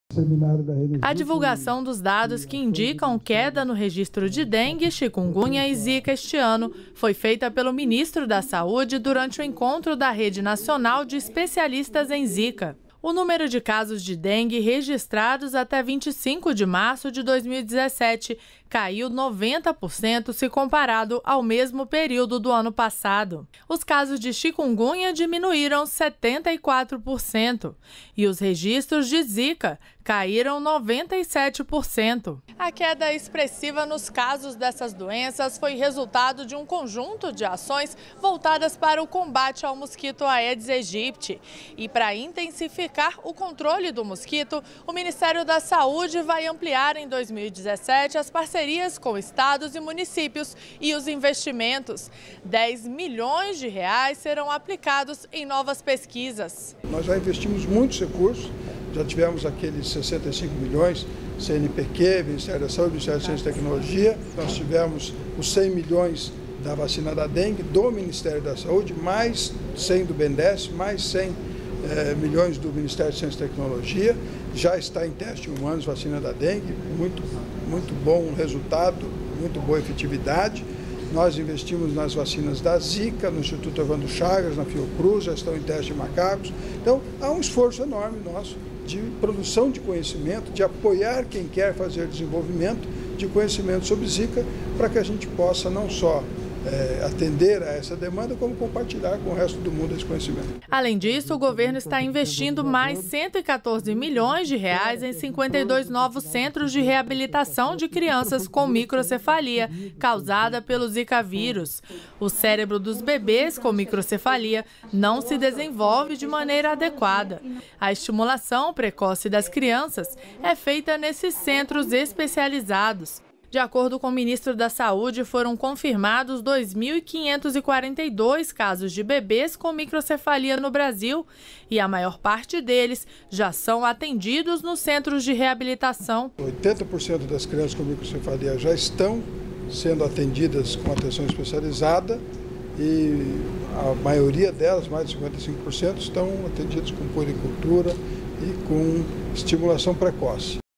Governo investirá R$ 10 milhões em pesquisas sobre doenças transmitidas pelo Aedes aegypti
REPÓRTER NBR - 30.03.17: Nesta quinta-feira (30), o Ministério da Saúde anunciou investimentos de R$ 10 milhões para pesquisas, além de ampliar parcerias com estados e municípios no combate ao inseto Aedes aegypti. Governo também investirá cerca de R$ 114 milhões em 52 novos centros de reabilitação para crianças com microcefalia causada pelo zika vírus.